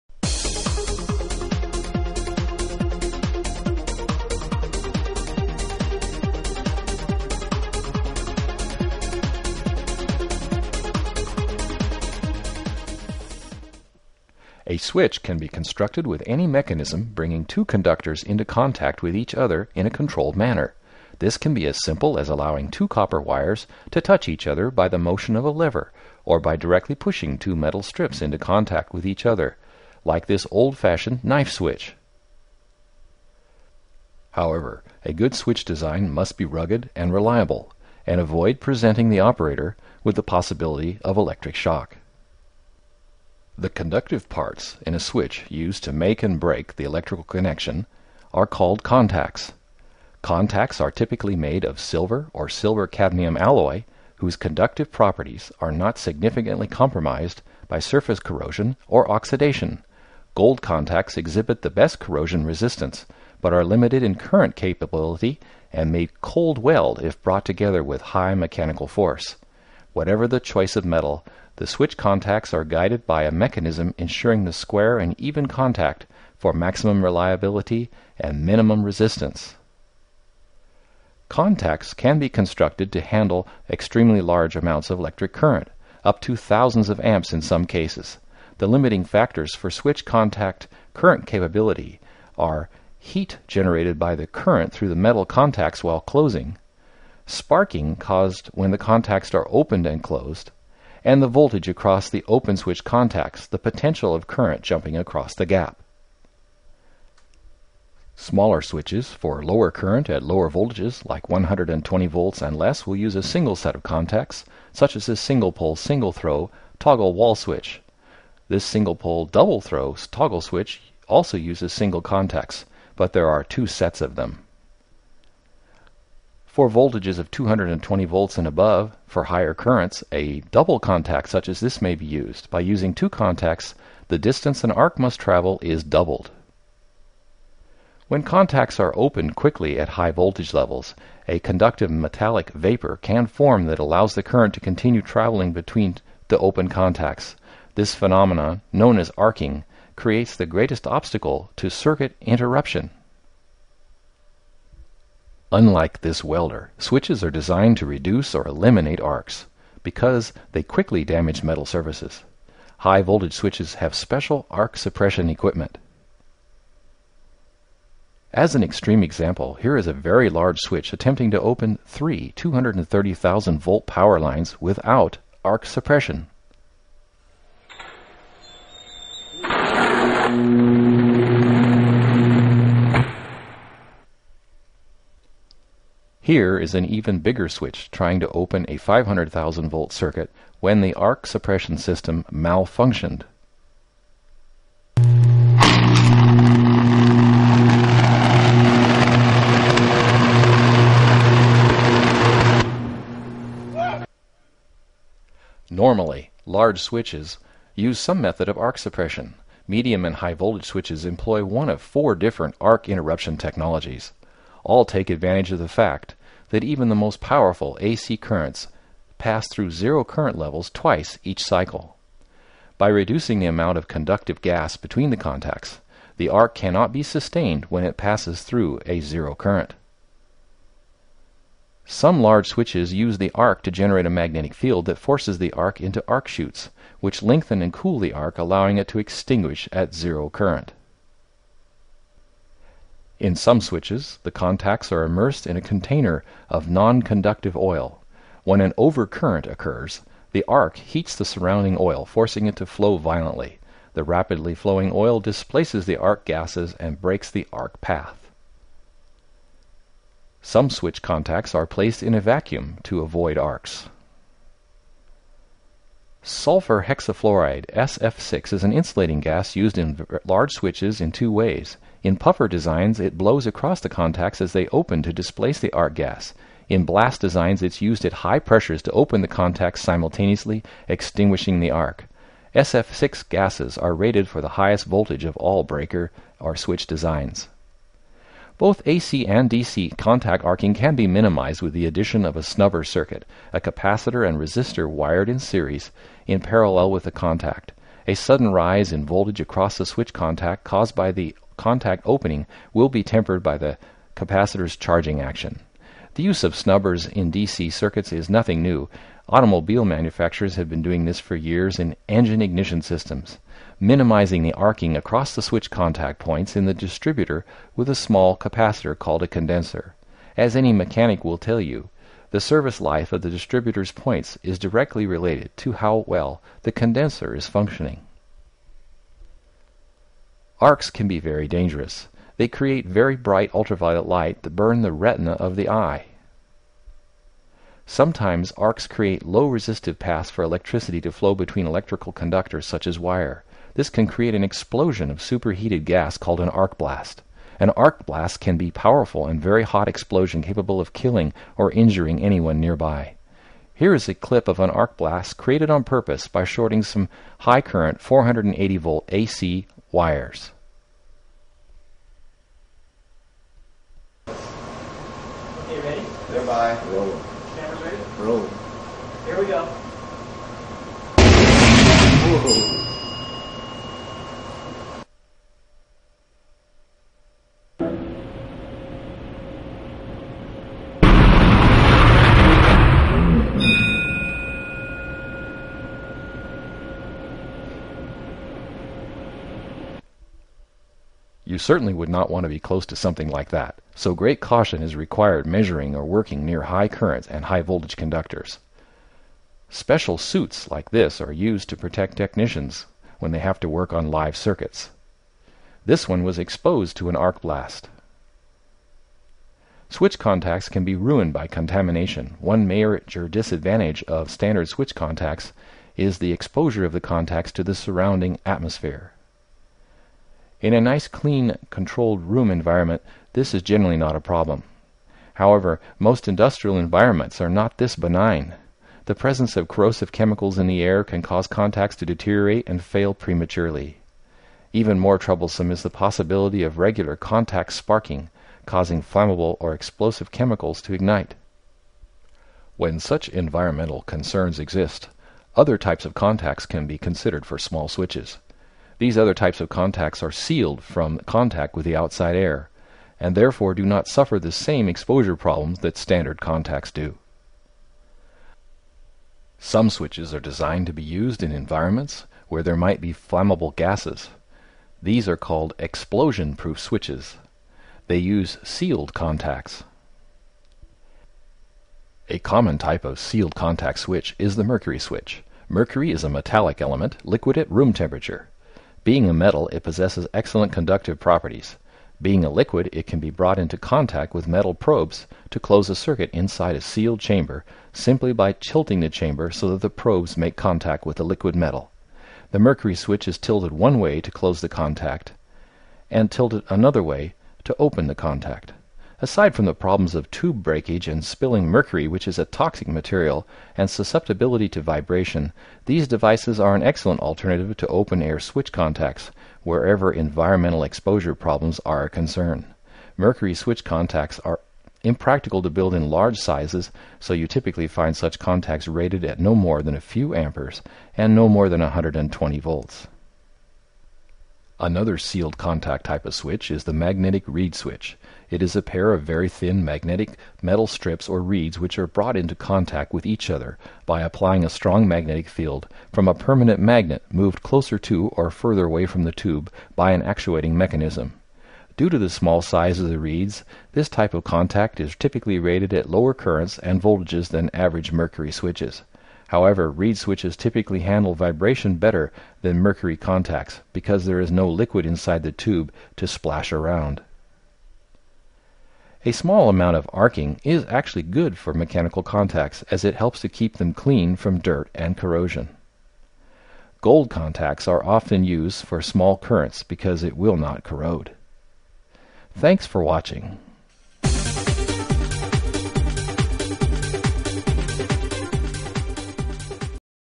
Electrical Switches, Part 5, Switch Contacts and Arcing
This video explains the importance of contacts in electrical switches. It describes the effects of arcing and contamination on switch operation.  It also shows the effects of high voltage arcs and the danger of arc blasts.